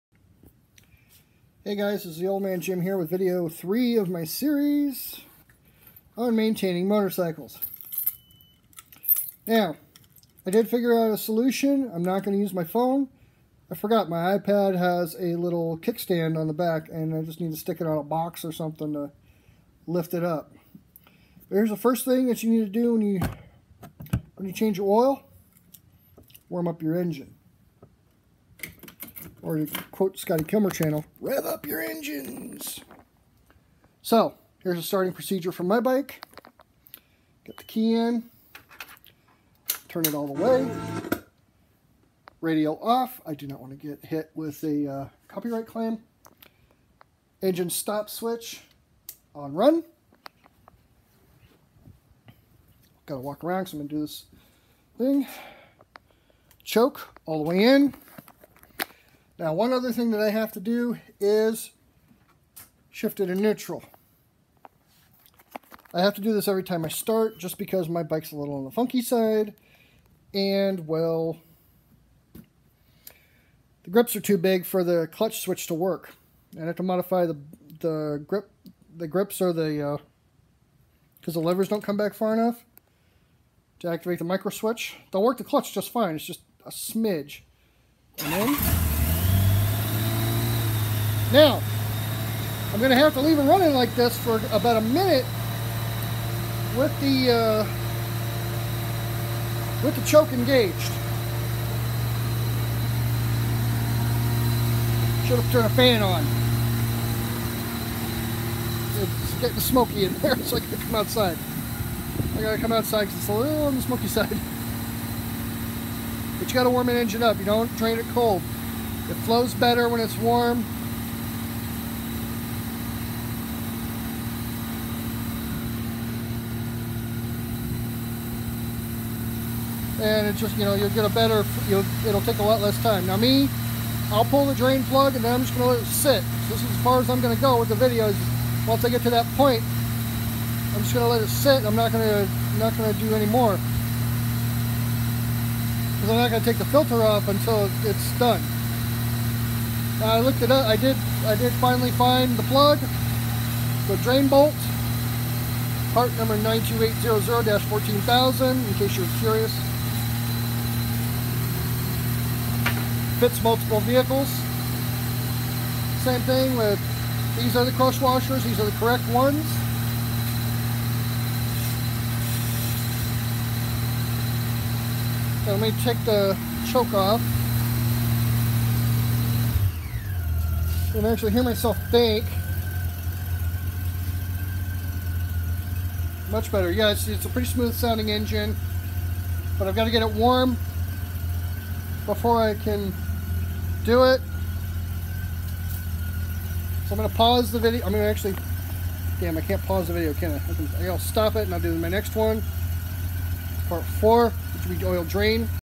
Part 3
Part 3 of my maintenance series. Shows you how much time and money you save doing it yourself.